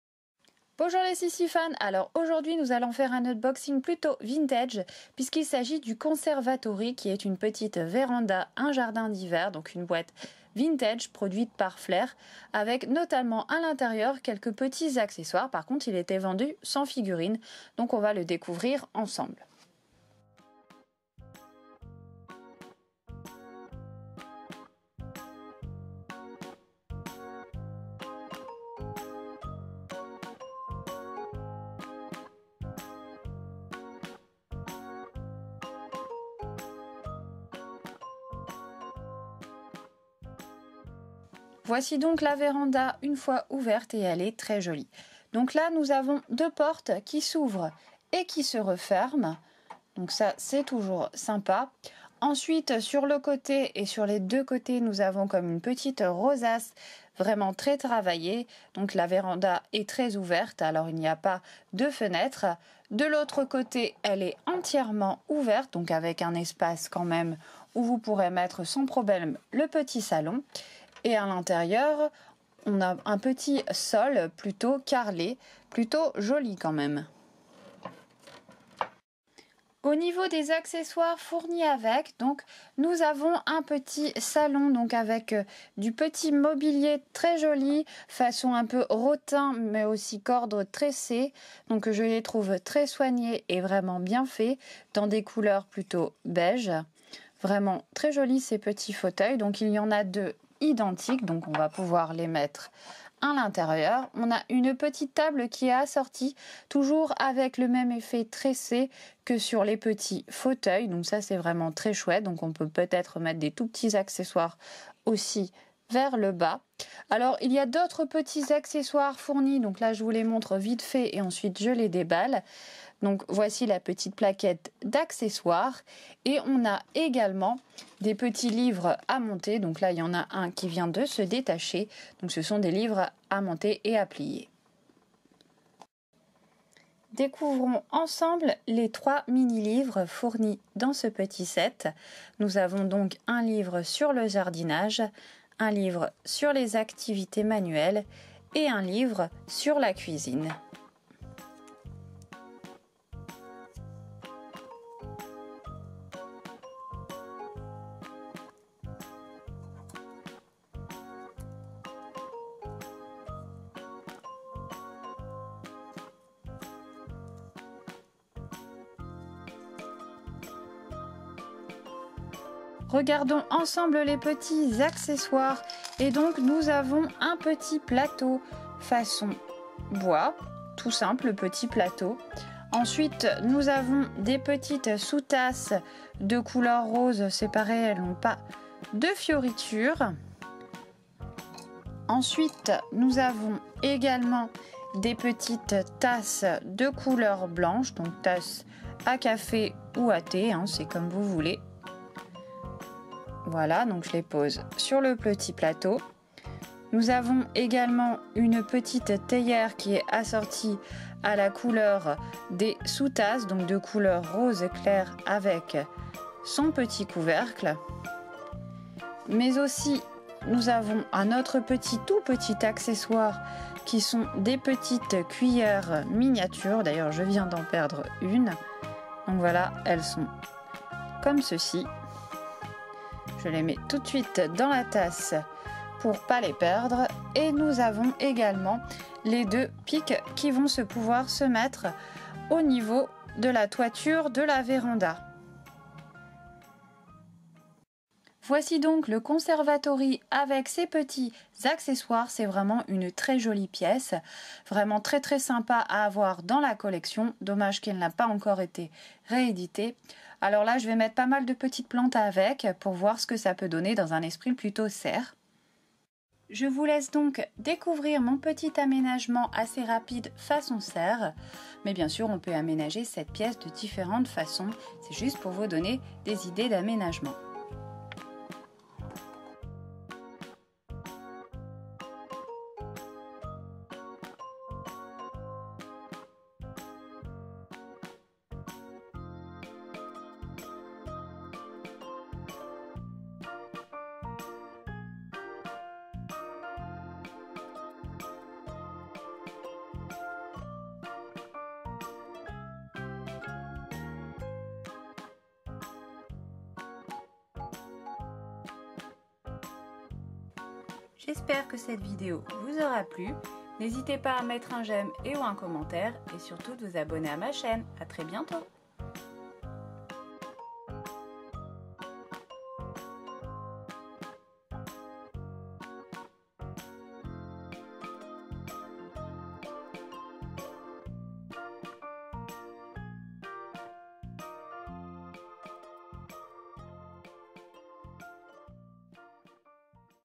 UNBOXING veranda / jardin d'hiver CONSERVATORY des SYLVANIAN FAMILIES FLAIR
Aujourd'hui dans cette vidéo je vous présente en UNBOXING une boite vintage qui est le conservatory (veranda/ jardin d'hiver) de FLAIR, un modèle vintage qui ajoute une pièce complémentaire aux maison Sylvanian Families (calico critters)

Et aussi sur le groupe facebook pour partager vos photos et bons plans